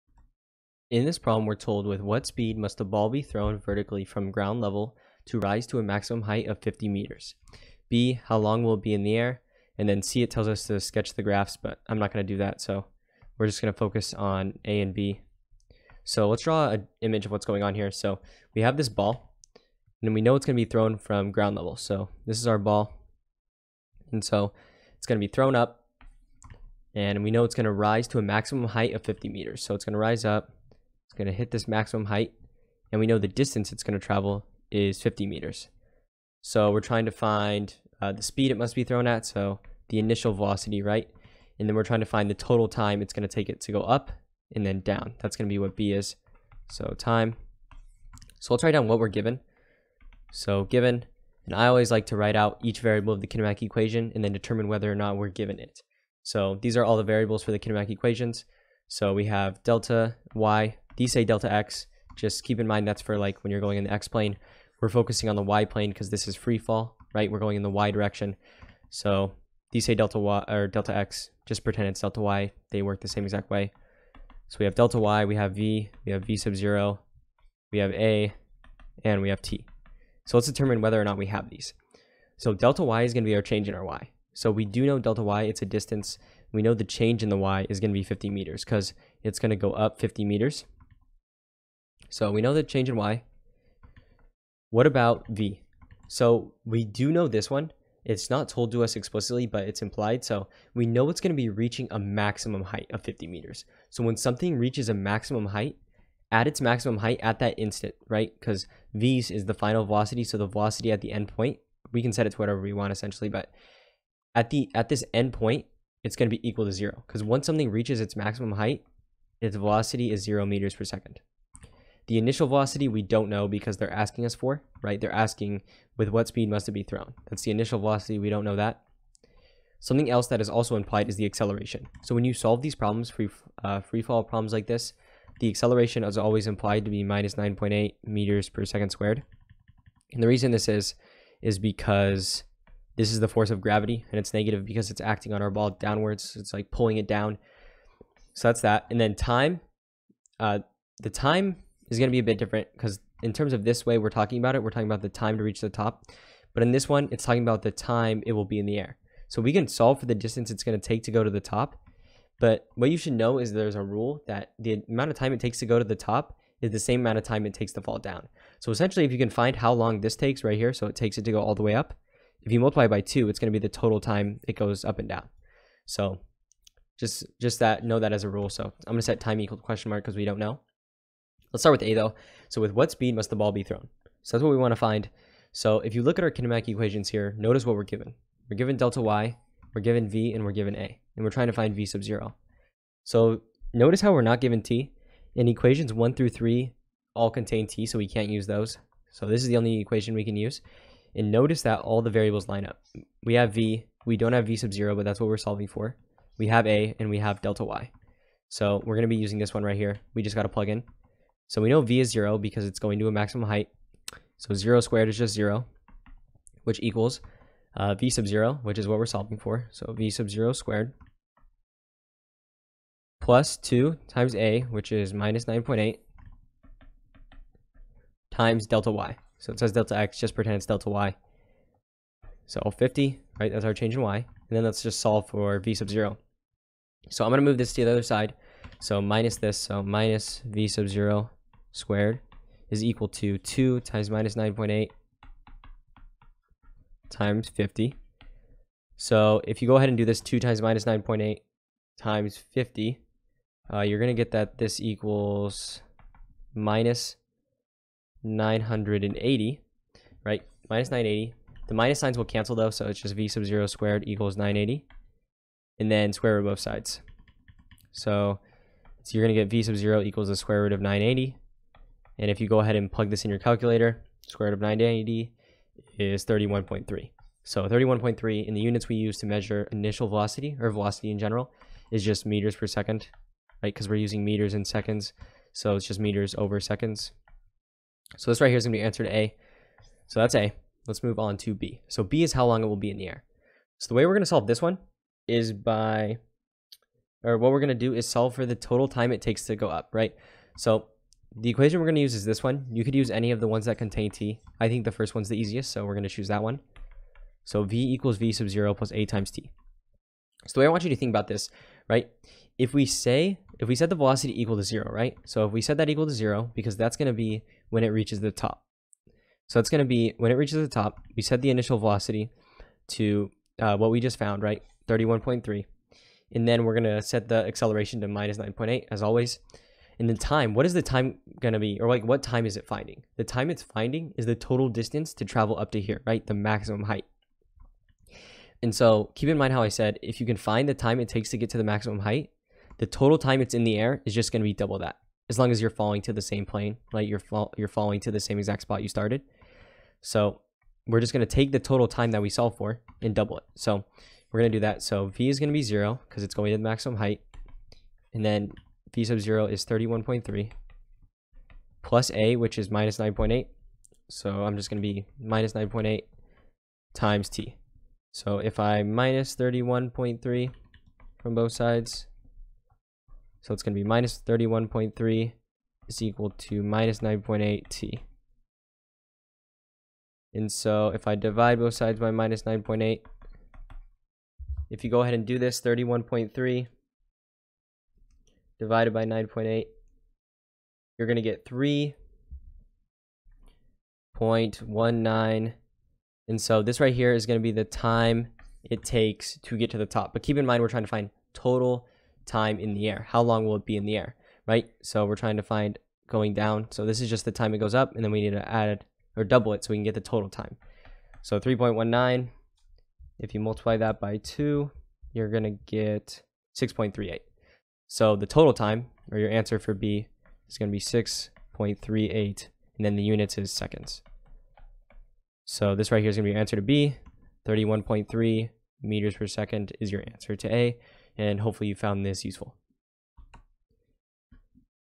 (a) With what speed must a ball be thrown vertically from ground level to rise to a maximum height
(a) With what speed must a ball be thrown vertically from ground level to rise to a maximum height of 50 m?
(b) How long will it be in the air? (c) Sketch graphs of y, v, and a
versus t for the ball. On the first two graphs, indicate the time at
which 50 m is reached. .